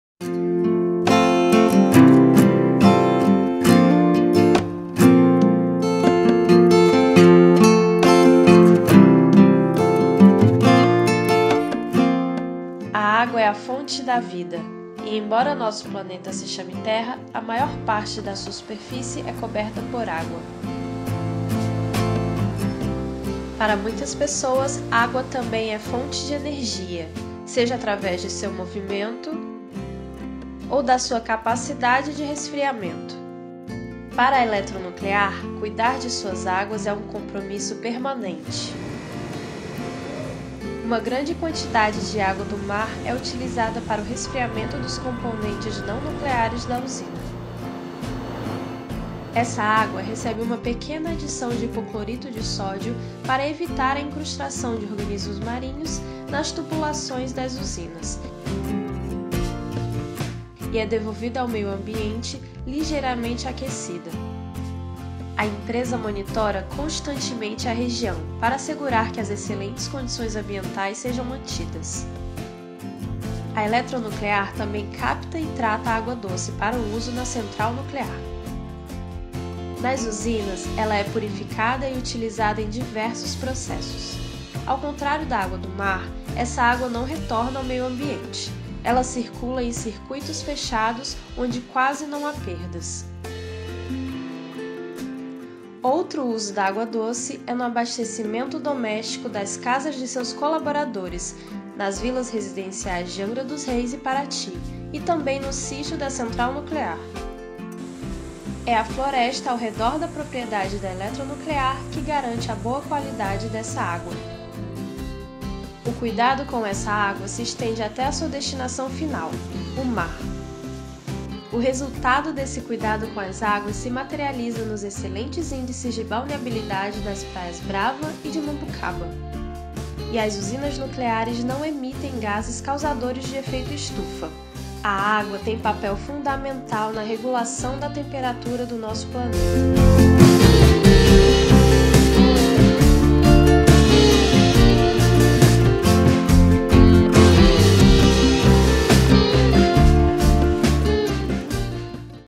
As águas da Eletronuclear
A água é a fonte da vida, e embora nosso planeta se chame Terra, a maior parte de sua superfície é coberta por água. Nesse vídeo, você vai entender melhor a importância dessa substância e como ela é utilizada em toda a central nuclear de Angra. A Eletronuclear se preocupa e tem responsabilidade com toda a água que utiliza ou que se encontra no entorno das usinas.